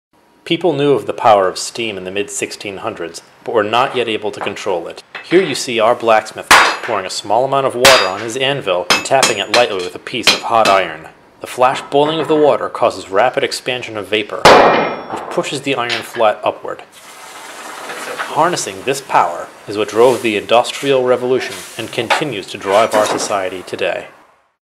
Flash-boiling water on an avil
A brief demonstration on how steam power works.